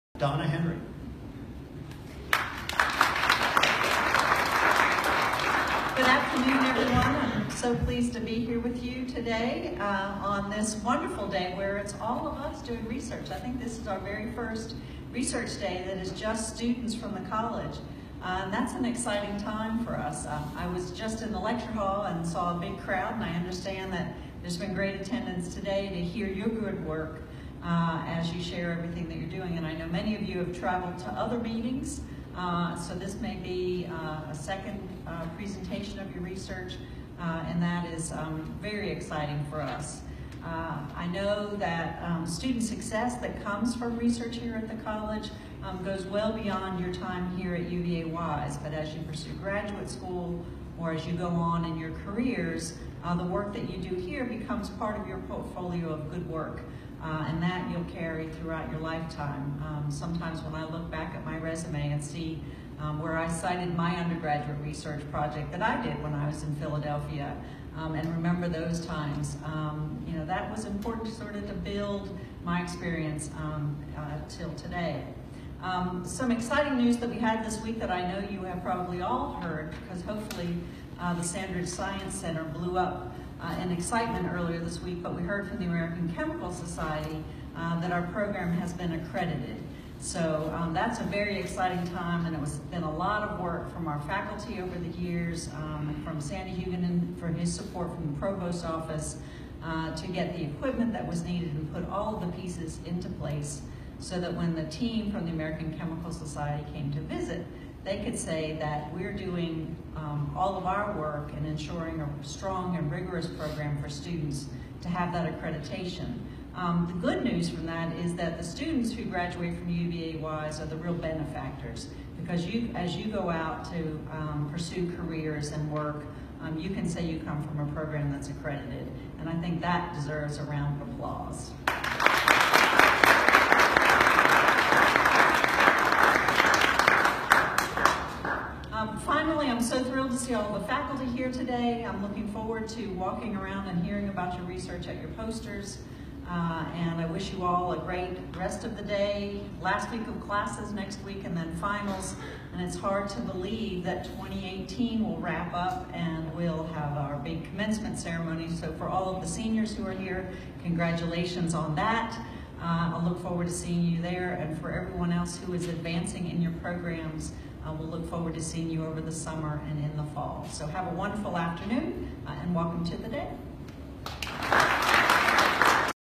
UVa-Wise Research Day 2018 Chancellor Henry speaking
Chancellor Henry addressing the crowd in the Leonard J. Sandridge Science Center for Research Day 2018!